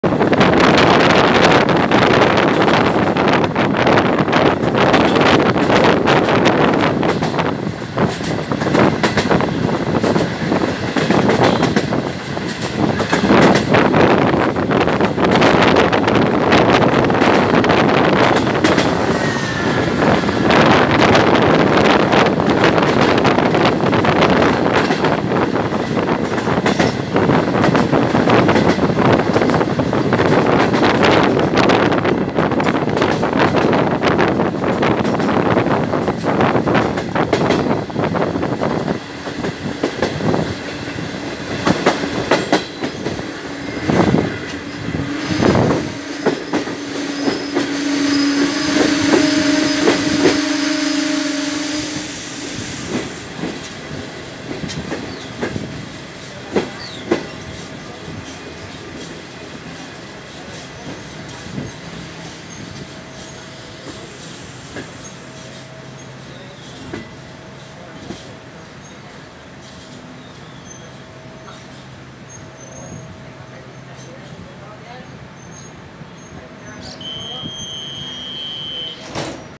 Cruzada con la SDD7 B955 y llegada a Va. del Parque (ventanilla)
Desde la ventanilla de una formación descendente vemos la cruzada con la CSR SDD7 B955 y la llegada a la estación de Villa del Parque, de la línea San Martín, el jueves 20 de febrero de 2020 a las 16:27 hs.